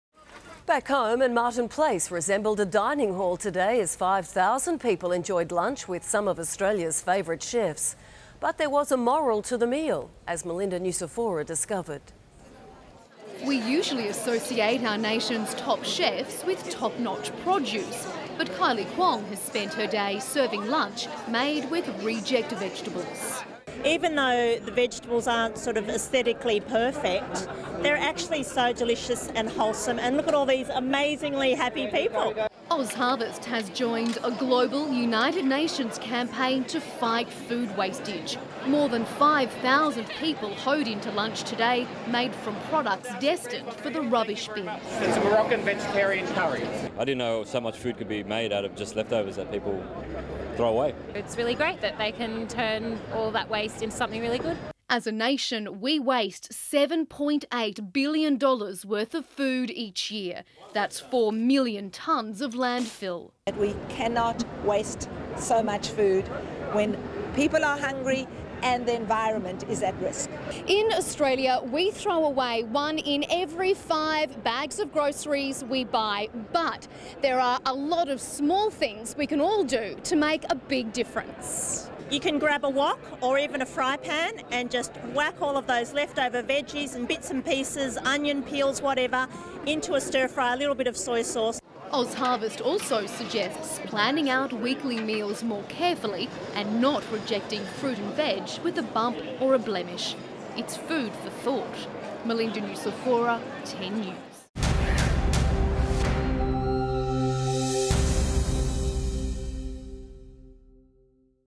From bin to plate
Mon 29/07/2013: Today 5000 people enjoyed lunch with some of Australia's favourite chefs, serving up meals made from produce destined for the rubbish bin